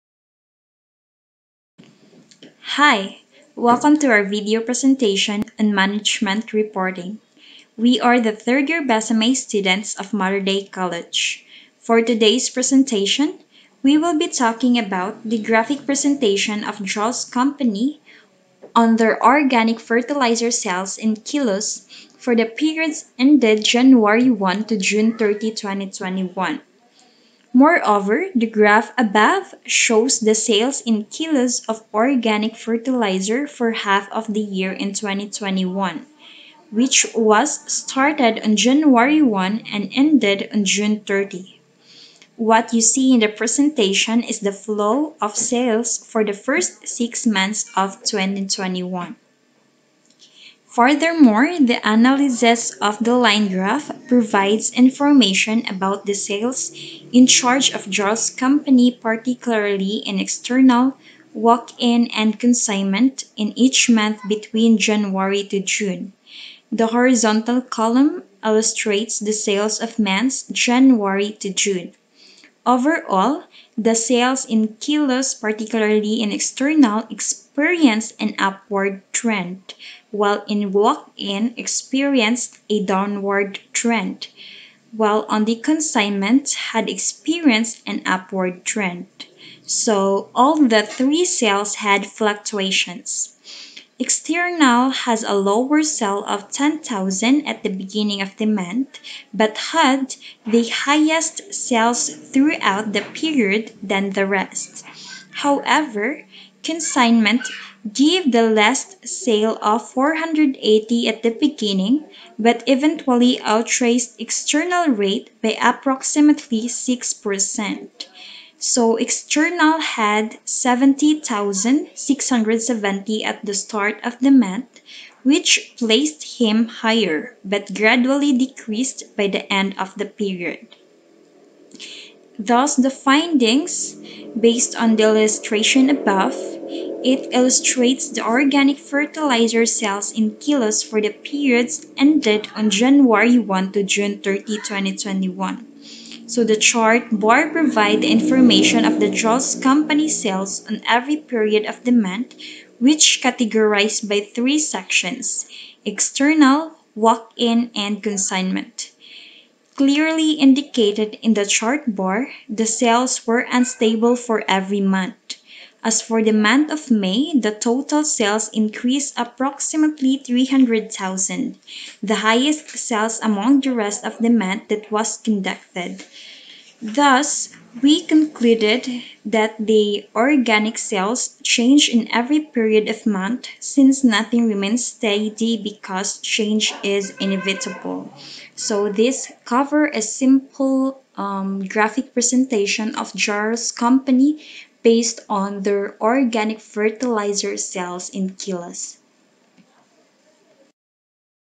MGTREPORT GROUP I: JARLS FERTILIZER SALES REPORTING
This class task aims to highlight the skills of the students in management reporting. From the traditional narrative sales reporting, the students were asked to prepare reports, analysis and come up with recommendations concerning the sales data given. In this task, the students were able to see and understand the different angles of simple sales data.